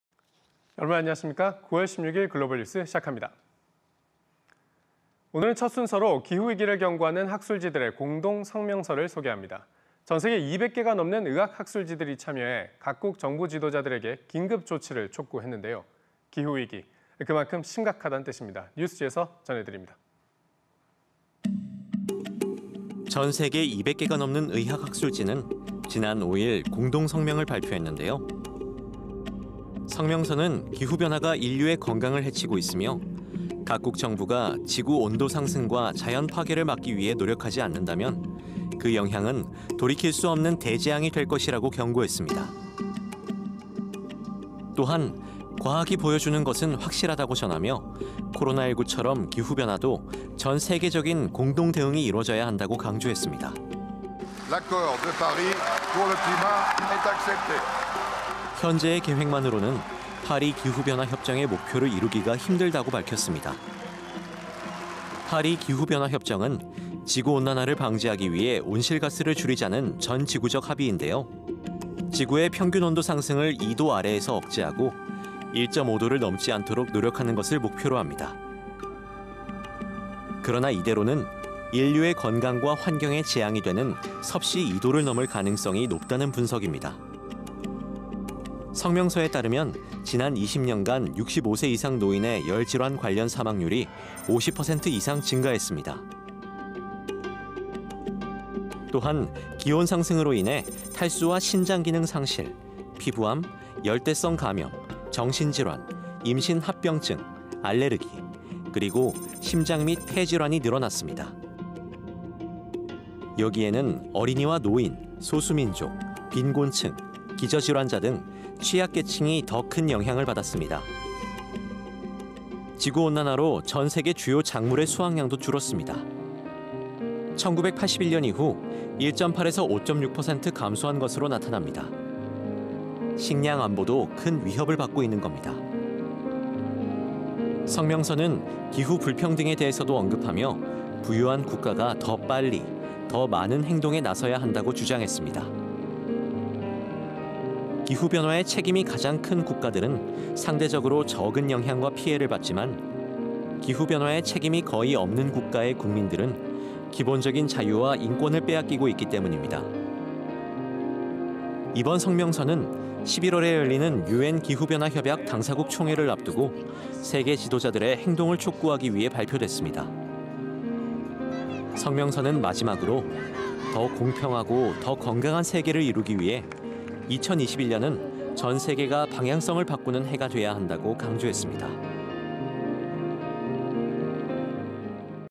2021. 09. 16. [뉴스g] 공공 보건을 위협하는 기후 변화
[EBS 뉴스G]

오늘은 첫 순서로 기후 위기를 경고하는 학술지들의 공동성명서를 소개합니다.

전 세계 200개가 넘는 의학 학술지들이 참여해 각국 정부 지도자들에게 긴급 조치를 촉구했는데요.

기후 위기, 그만큼 심각하다는 뜻입니다.

뉴스G에서 전해드립니다.

[리포트]

전 세계 200개가 넘는 의학학술지는 지난 5일, 공동 성명을 발표했는데요,

성명서는 기후 변화가 인류의 건강을 해치고 있으며, 각국 정부가 지구 온도 상승과 자연 파괴를 막기 위해 노력하지 않는다면, 그 영향은 돌이킬 수 없는 '대재앙'이 될 것이라고 경고했습니다.

또한, 과학이 보여주는 것은 확실하다고 전하며, 코로나19처럼 기후 변화도 전 세계적인 공동 대응이 이뤄져야 한다고 강조했습니다.

현재의 계획만으로는 파리 기후변화협정의 목표를 이루기가 힘들다고 밝혔습니다.

파리 기후변화협정은 지구온난화를 방지하기 위해 온실가스를 줄이자는 전 지구적 합의인데요.

지구의 평균 온도 상승을 2도 아래에서 억제하고, 1.5도를 넘지 않도록 노력하는 것을 목표로 합니다.

그러나 이대로라면 인류의 건강과 환경에 재앙이 되는 섭씨 2도를 넘을 가능성이 높다는 분석입니다.

성명서에 따르면 지난 20년간 65세 이상 노인의 열 질환 관련 사망률이 50% 이상 증가했습니다.

또한, 기온 상승으로 인해 탈수와 신장 기능 상실, 피부암, 열대성 감염, 정신 질환, 임신 합병증, 알레르기, 그리고 심장 및 폐 질환이 늘어났습니다.

여기에는 어린이와 노인, 소수민족, 빈곤층, 기저질환자 등 취약 계층이 더 큰 영향을 받았습니다.

지구 온난화로 전 세계 주요 작물의 수확량도 줄었습니다.

1981년 이후 1.8~5.6% 감소한 것으로 나타납니다.

식량안보도 큰 위협을 받고 있는 겁니다.

성명서는 '기후 불평등'에 대해서도 언급하며, 부유한 국가가 더 빨리, 더 많은 행동에 나서야 한다고 주장했습니다.

기후 변화에 책임이 가장 큰 국가들은 상대적으로 적은 영향과 피해를 받지만, 기후 변화에 책임이 거의 없는 국가의 국민들은 기본적인 자유와 인권을 빼앗기고 있기 때문입니다.

이번 성명서는 11월에 열리는 유엔기후변화협약 당사국총회를 앞두고 세계 지도자들의 행동을 촉구하기 위해 발표됐습니다.

성명서는 마지막으로, 더 공평하고 더 건강한 세계를 이루기 위해 2021년은 전 세계가 방향성을 바꾸는 해가 돼야 한다고 강조했습니다.